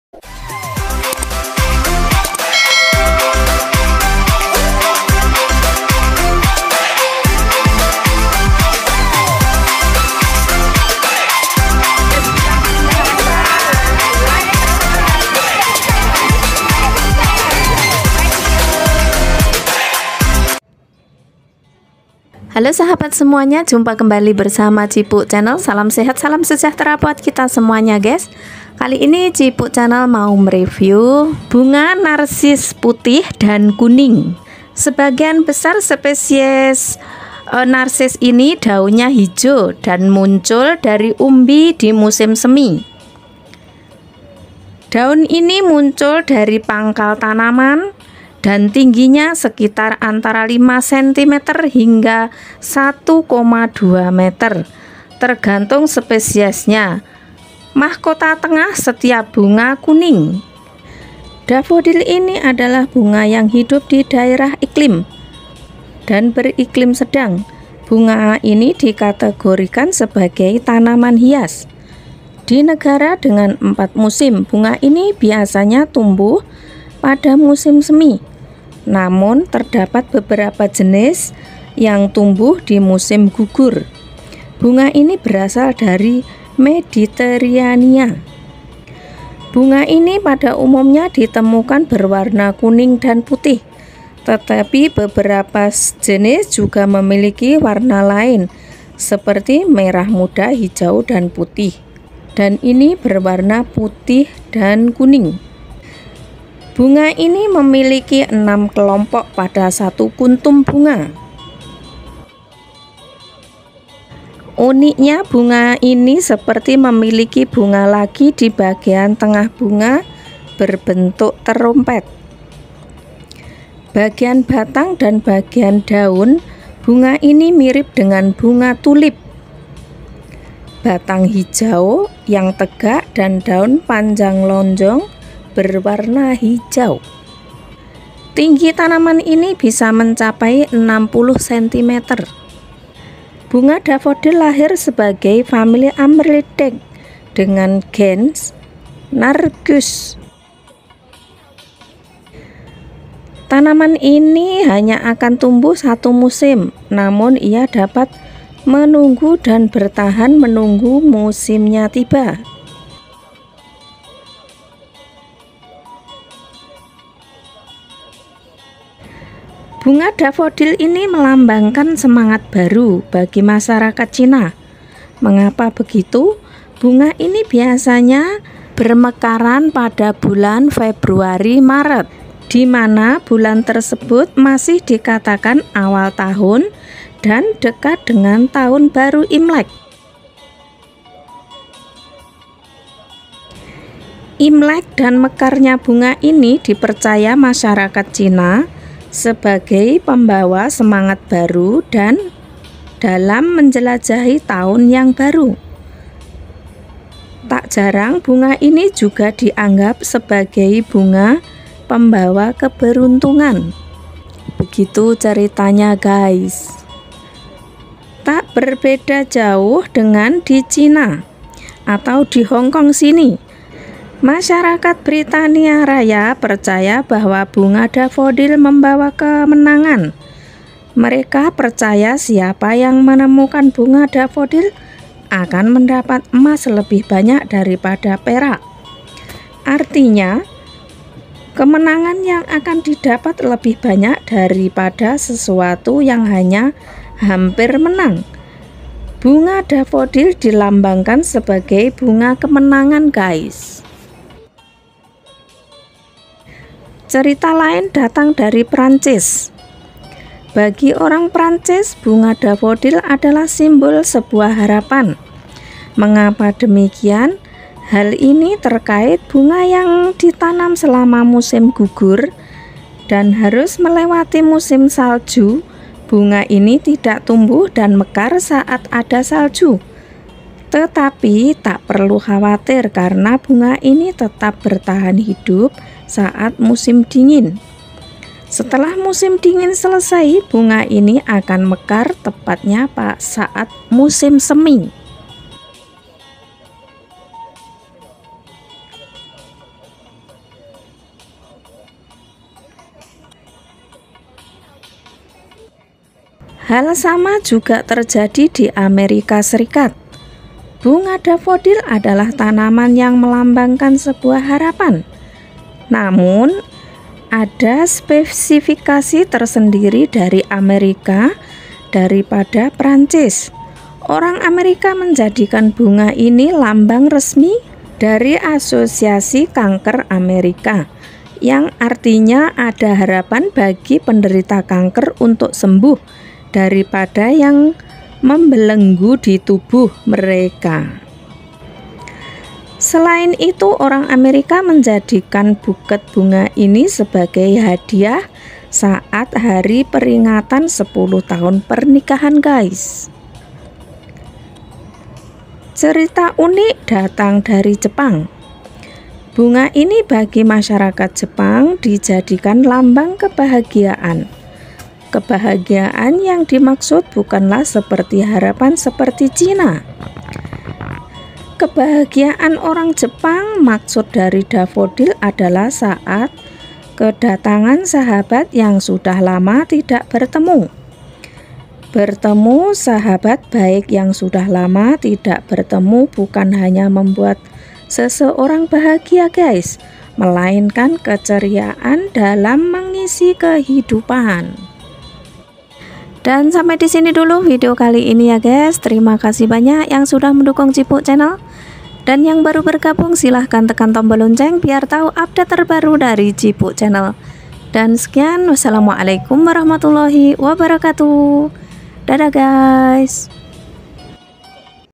BUNGA BAKUNG | NARCISSUS BUNGA HIAS
bunga bakung atau NARCISSUS tanaman ini mekar di awal hingga pertengahan musim semi
bunganya harum.tahan panas tahan kering tahan hama tahan pnyakitdan berwarna putih kuning
trimakasih banyak yang sudah mendukung channel q Dan yang baru menemukan channel q